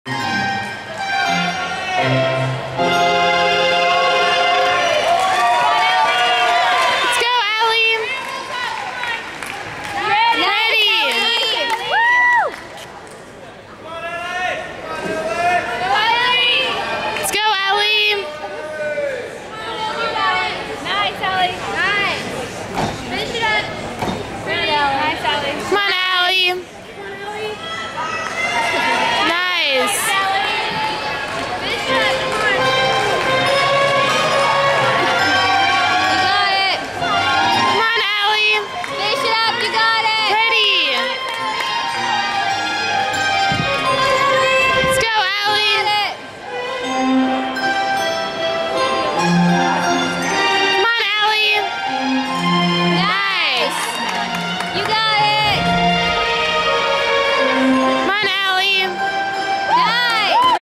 Elli Beam Comp State
Highland Park Girls Gymnastics
2015 State Championships - Compulsory
hosted by Chisholm Trail HS
5/1/15